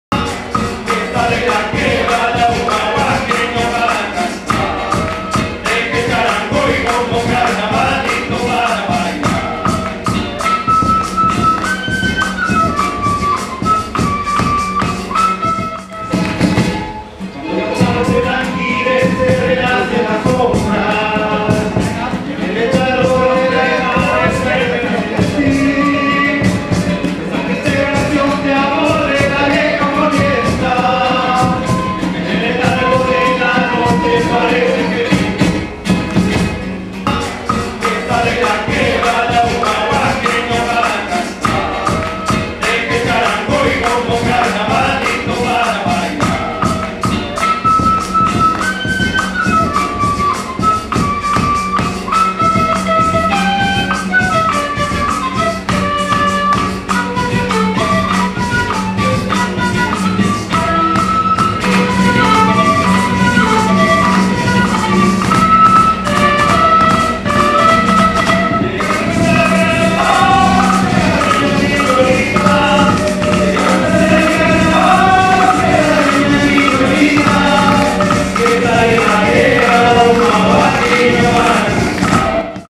Los Chasquis UID
Grupo de música andina en la Universidad Iberoamericana del Deporte